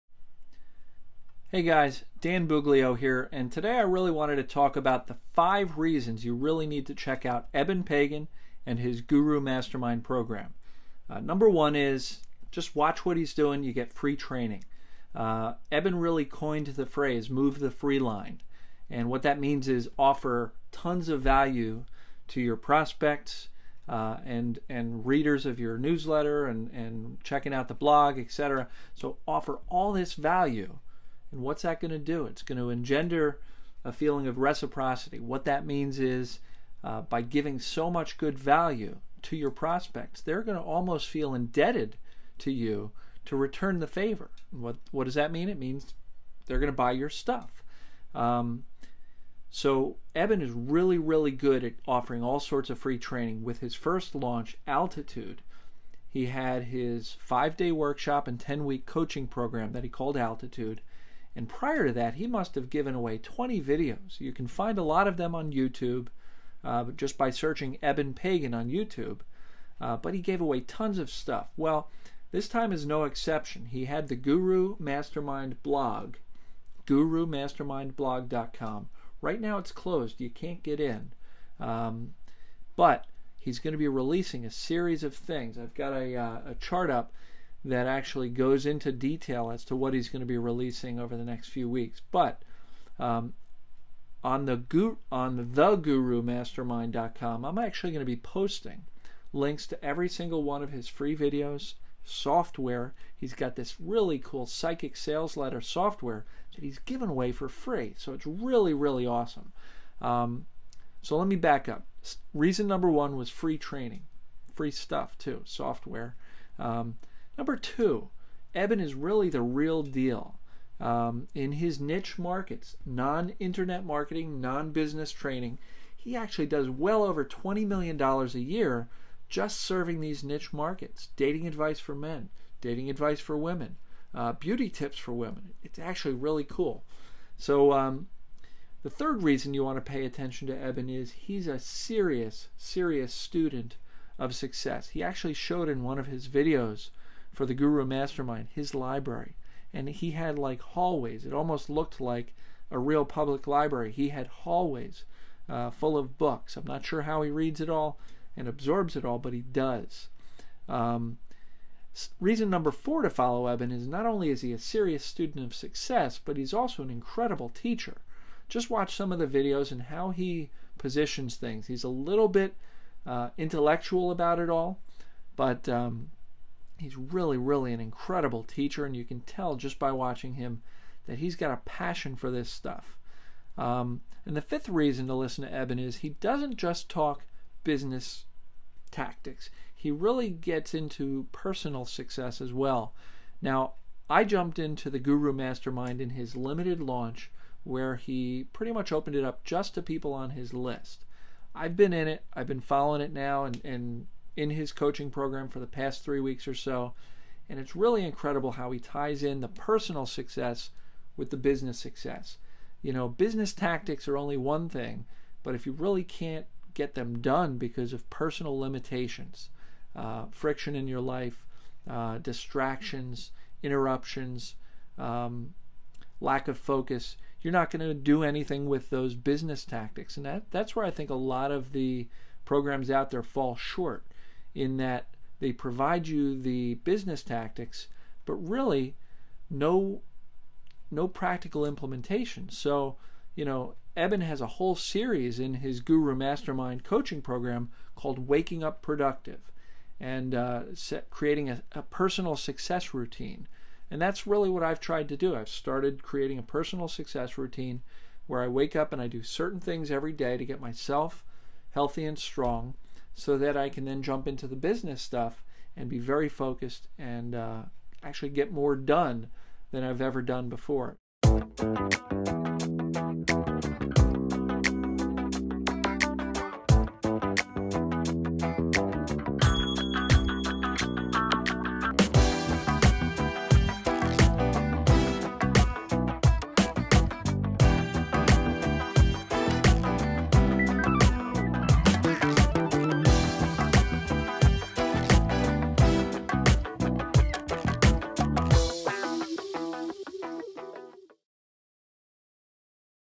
Eben Pagan - 5 Reasons to Listen
5 Reasons to tune in and pay very close attention to Eben Pagan and what he's doing with his Guru Mastermind Coaching Program.